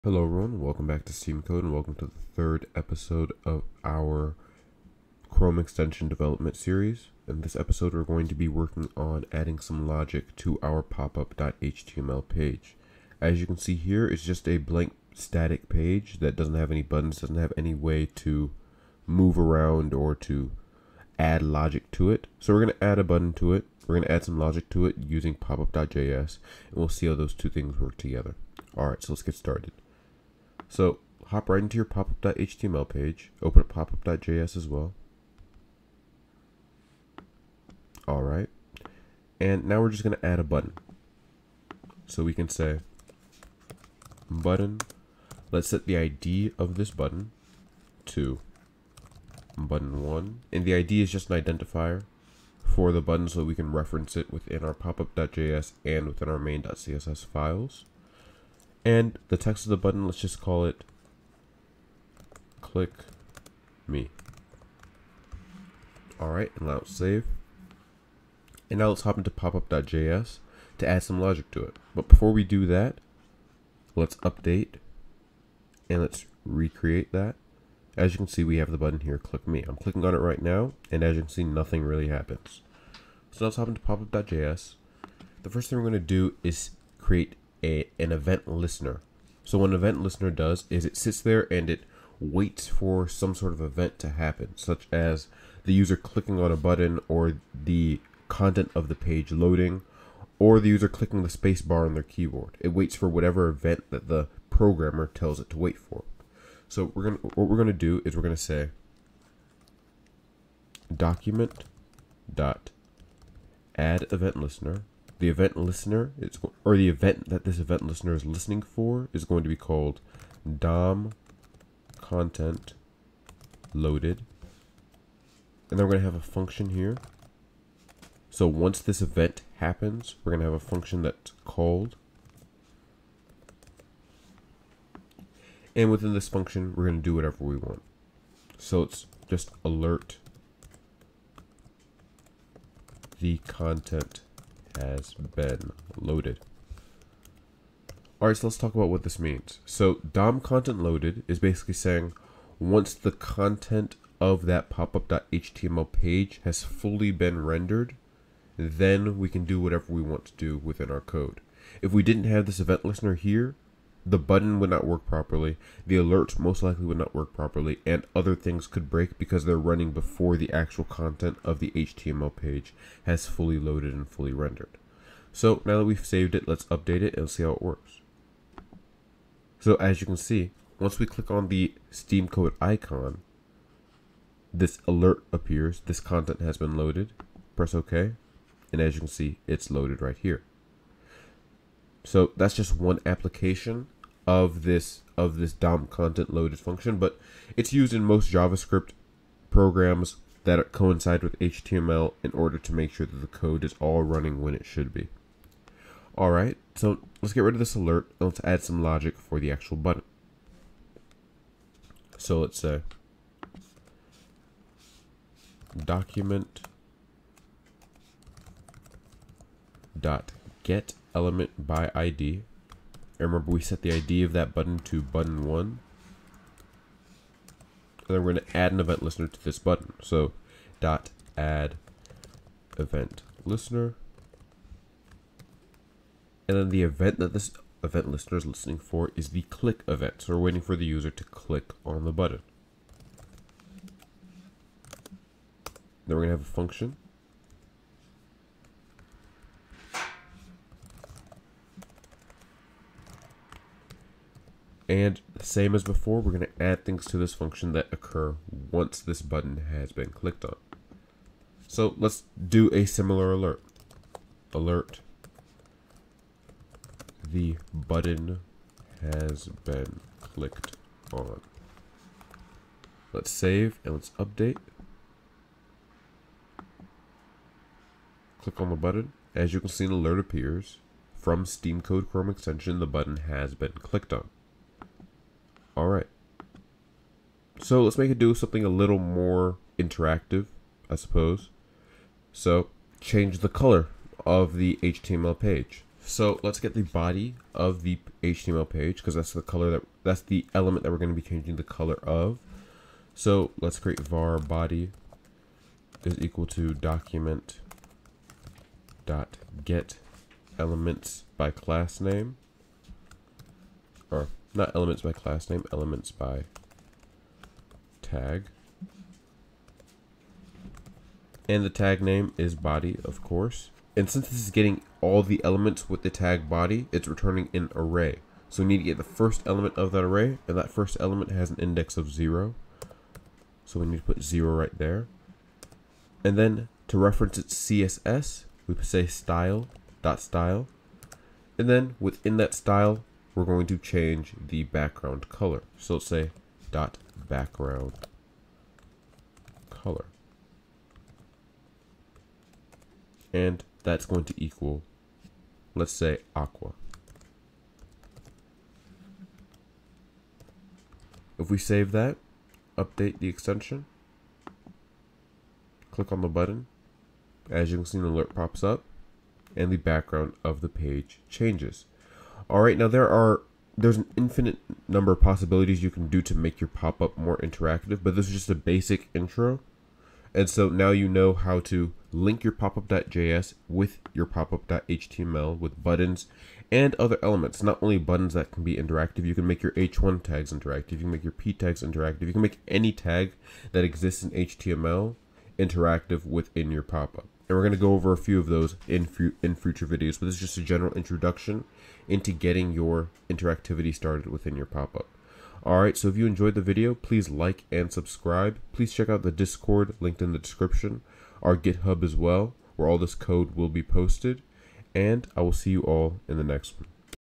Making a Chrome Extension: Part 3 (Popup Logic)
Hello everyone, welcome to SteamCode. In this part of the Chrome Extension Development series, I will be showing you how to add logic to your Chrome extension popup menu. If you have any questions, do not hesitate to leave them in the comment section down below or on Discord!